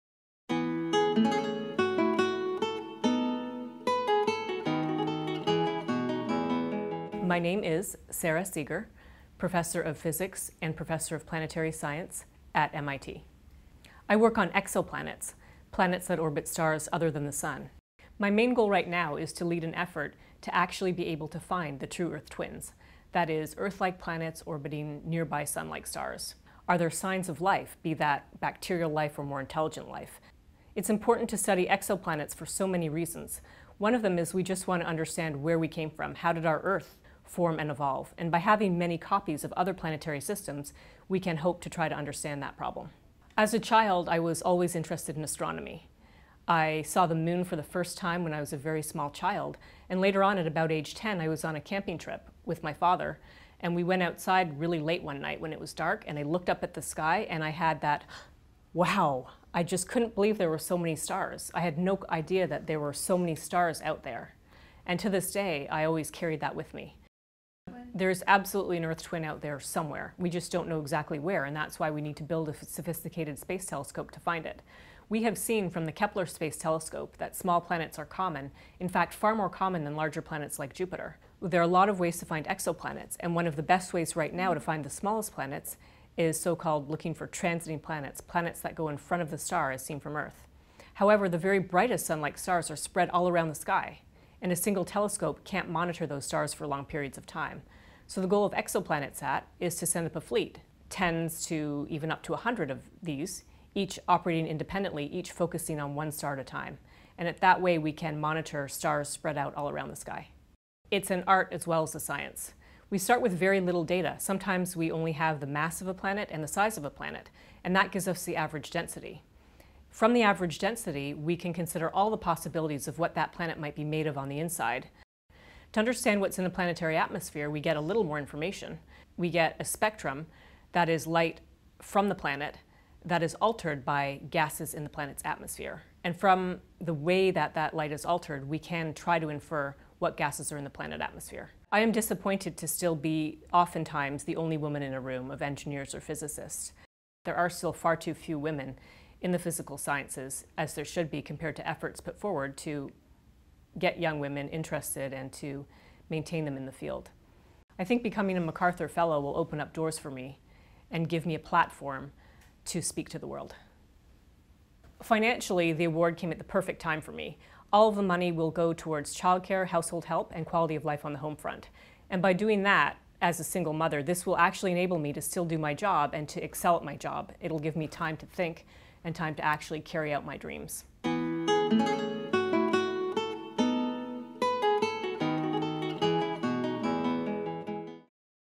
Astrophysicist Sara Seager, 2013 MacArthur Fellow | MacArthur Foundation
Astrophysicist Sara Seager was named a MacArthur Fellow in 2013. The Fellowship is a $625,000, no-strings-attached grant for individuals who have shown exceptional creativity in their work and the promise to do more.